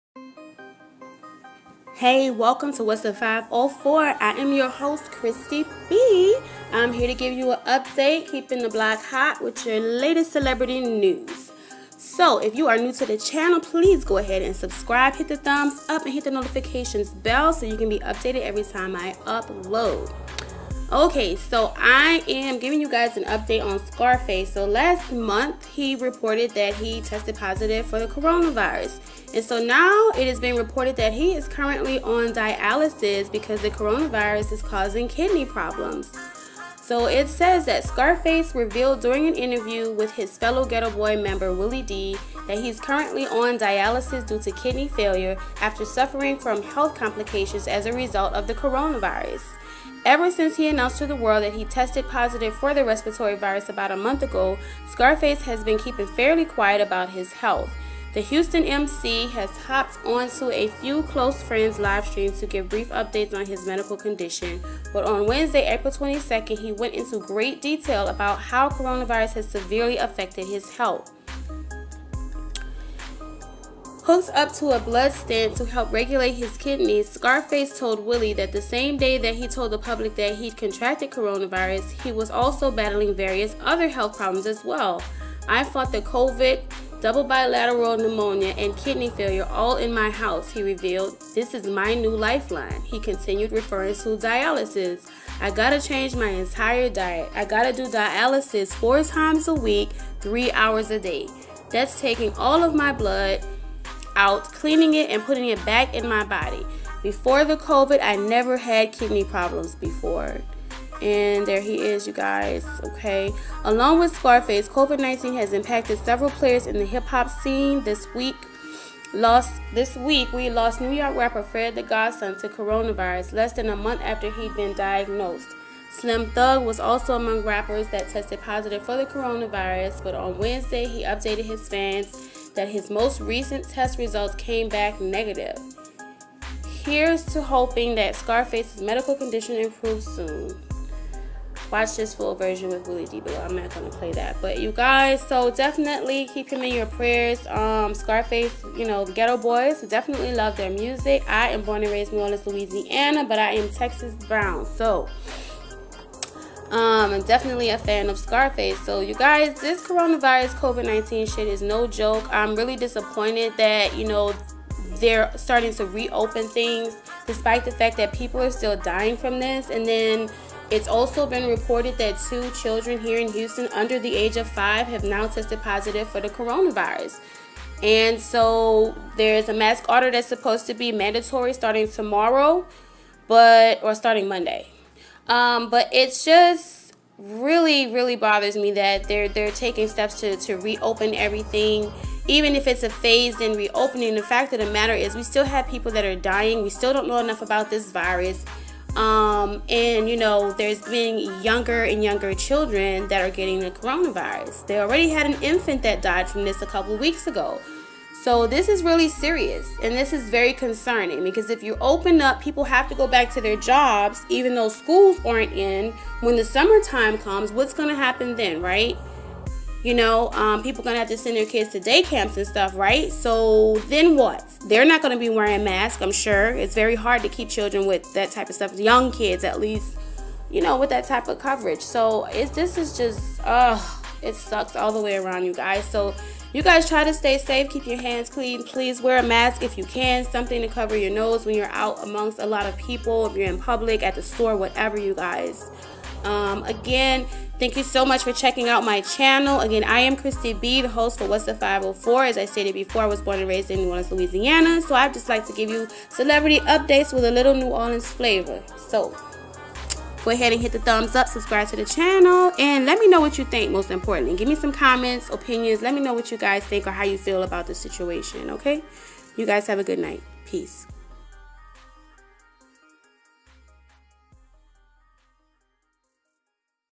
RAPPER SCARFACE ON DIALYSIS AFTER CORONAVIRUS DIAGNOSIS
RAPPER SCARFACE IS ON DIALYSIS TREATMENTS FOR HIS KIDNEYS DUE TO CORONAVIRUS COMPLICATIONS. SCARFACE CONFIRMED HE TESTED POSITIVE FOR THE CORONAVIRUS/COVID 19 ABOUT 3 WEEKS AGO. SINCE THEN HE HAS BEEN DEALING WITH MULTIPLE HEALTH ISSUES. TUNE IN FOR  DETAILS! THANKS FOR STOPPING BY! SMOOCHES!